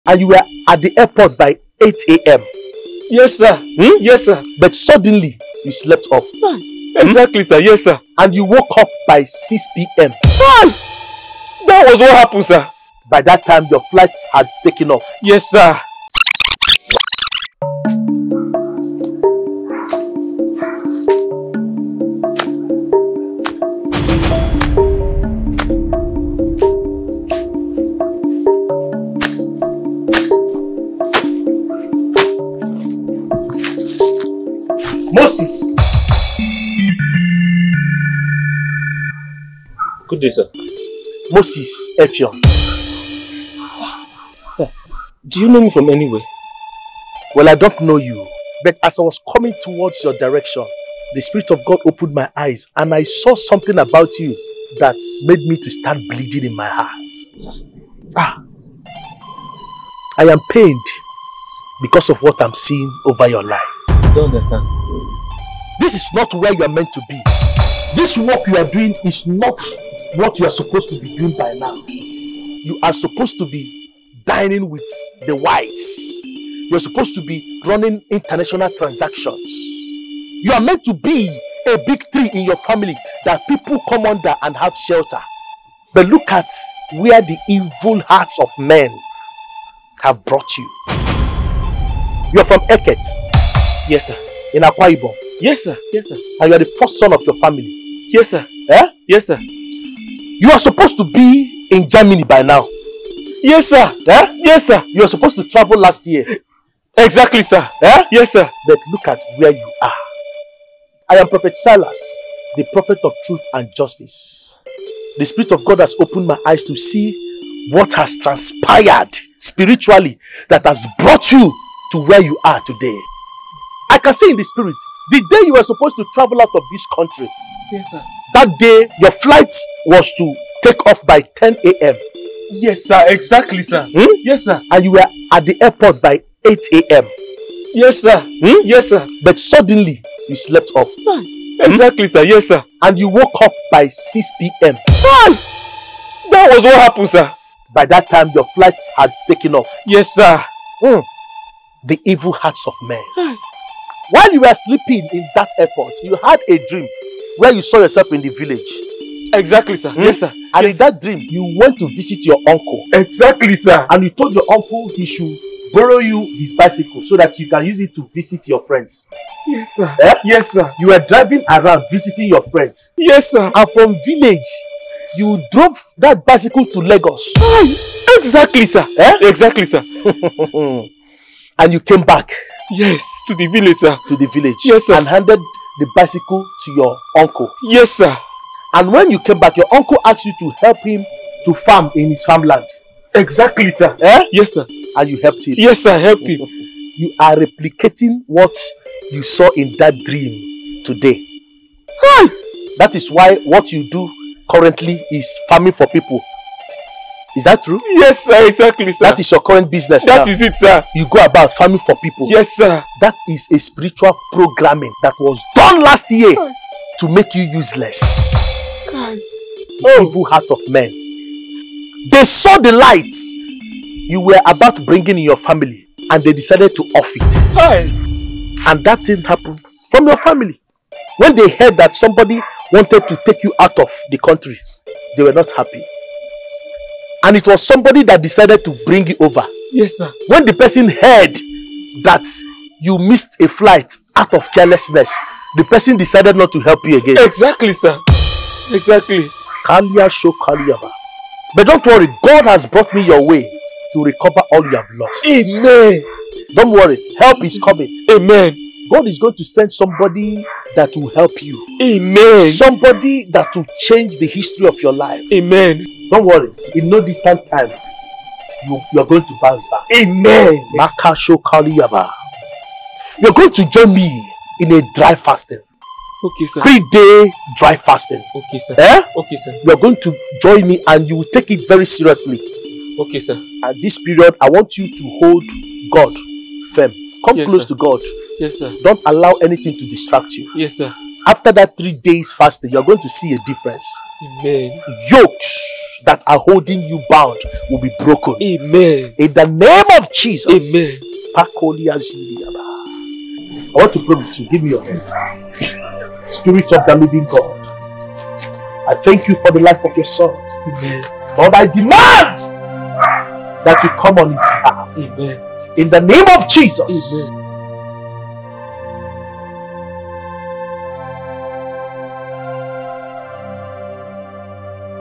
HE SLEPT OFF AT THE AIRPORT AND MISSED HIS FLIGHT TO GERMANY, SEE WHY - Prophet Silas 113
VILLAGE PEOPLE CAUSED HIM TO SLEEP OFF AND MISSED HIS FLIGHT TO GERMANY - Prophet Silas 113